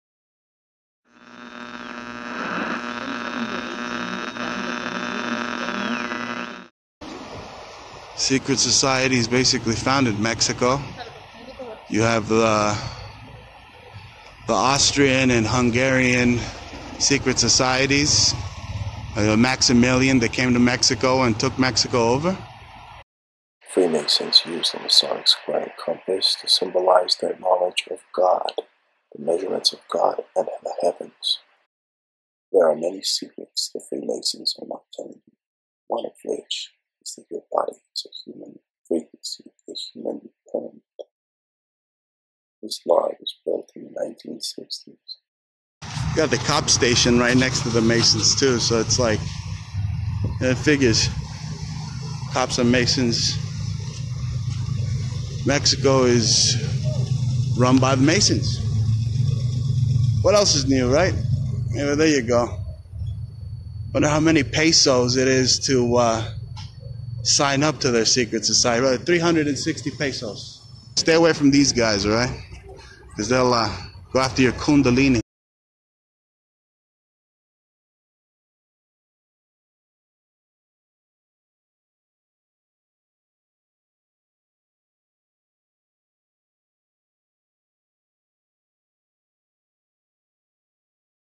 Bruce Montalvo Nogales Mexico Masonic Lodge
Bruce breaks the border down in Mexico exposing  all things Masonic.